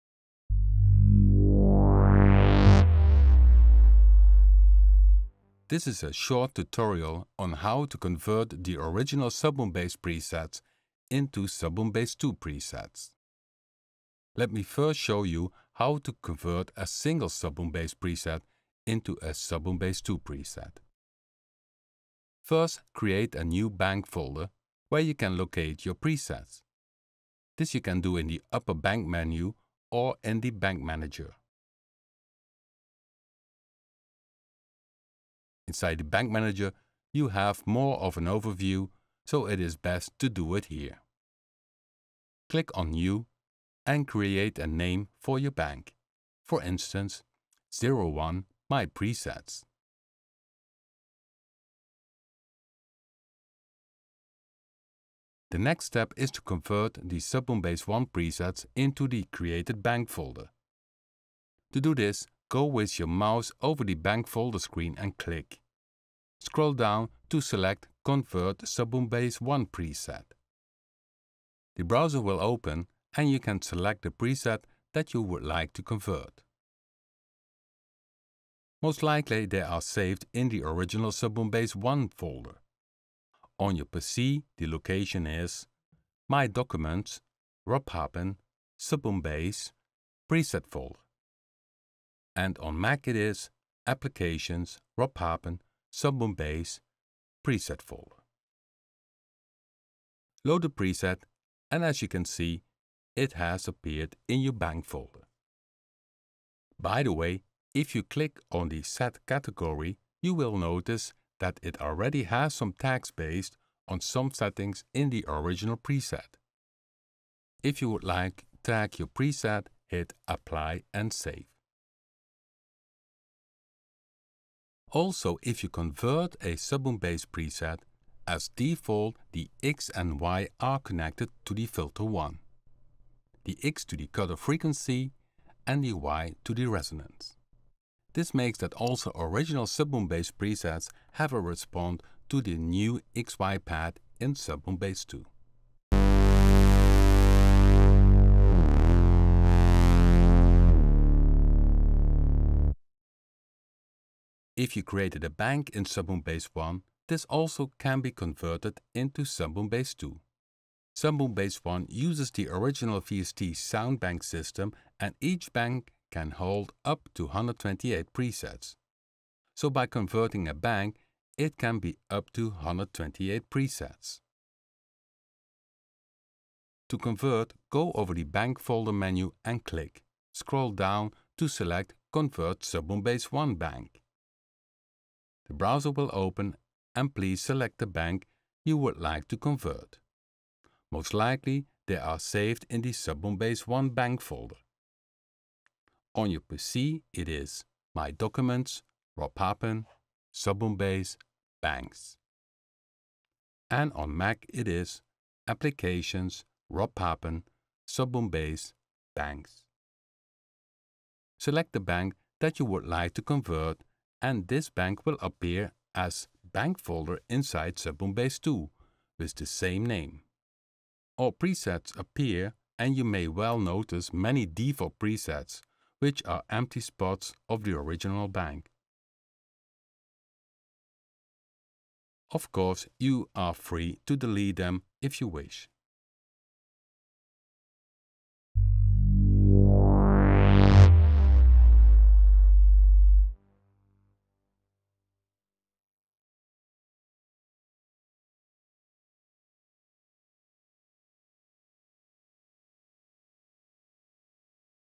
SubBoomBass2 Converting SubBoomBass presets
How to convert your own SubBoomBass presets into SubBoomBass 2.